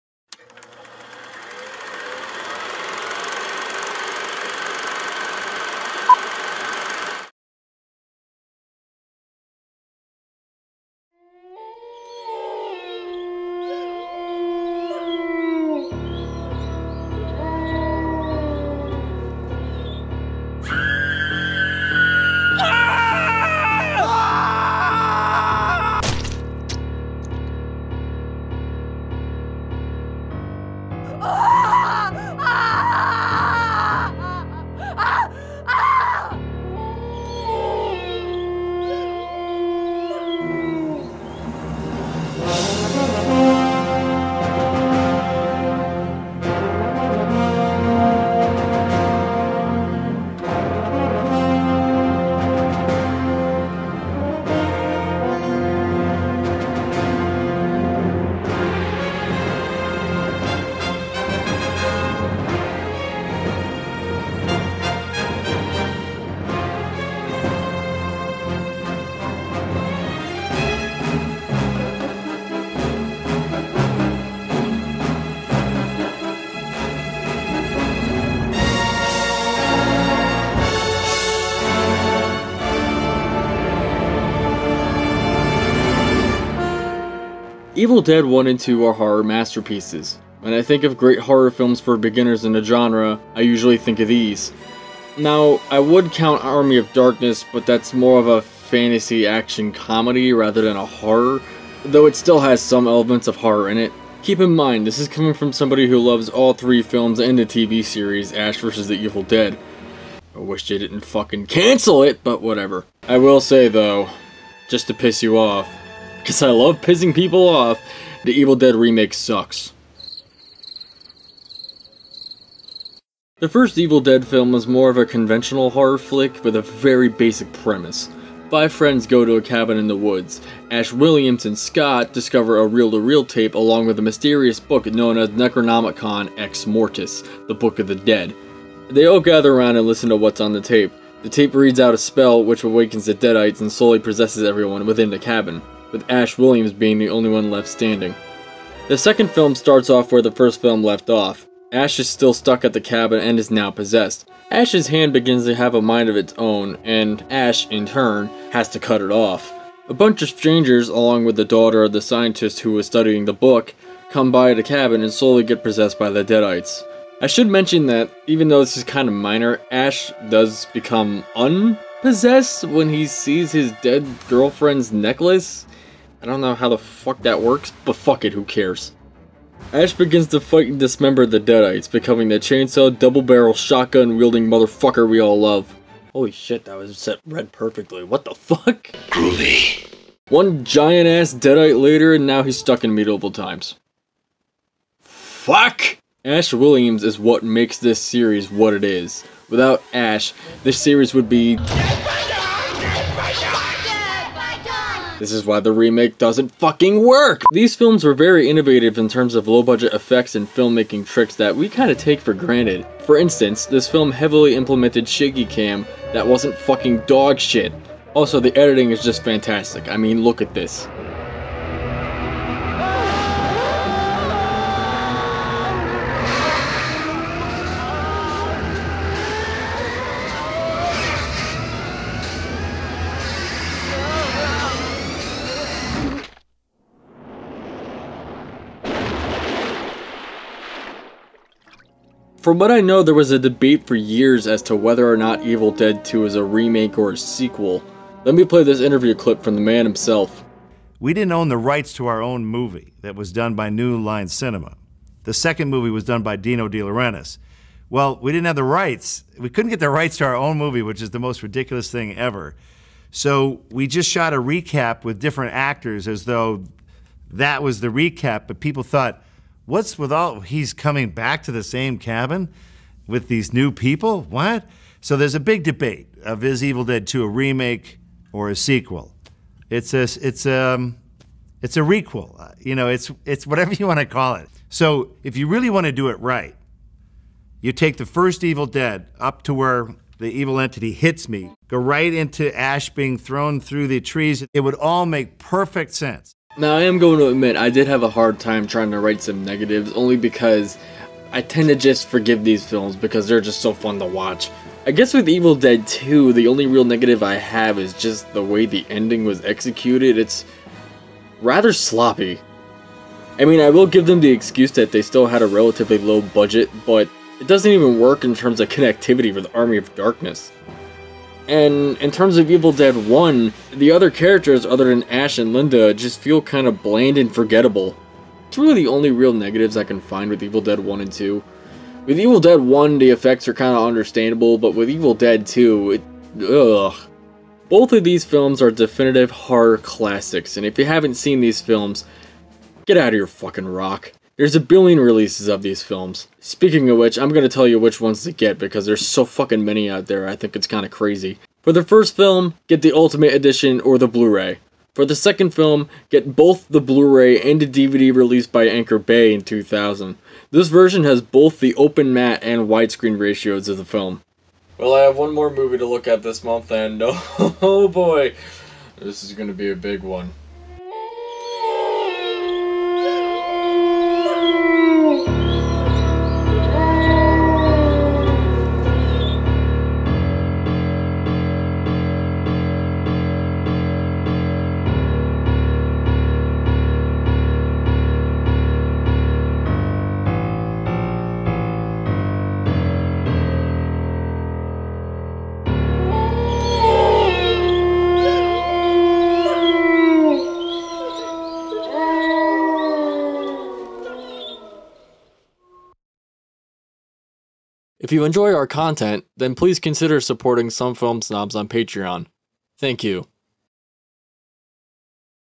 Snoboween (2018): Evil Dead I & II Review
In part 1 of our first Halloween special, Sean Rajnert takes a look at two cult classics you can cut your hands off to. Grab your book of the dead and take a look.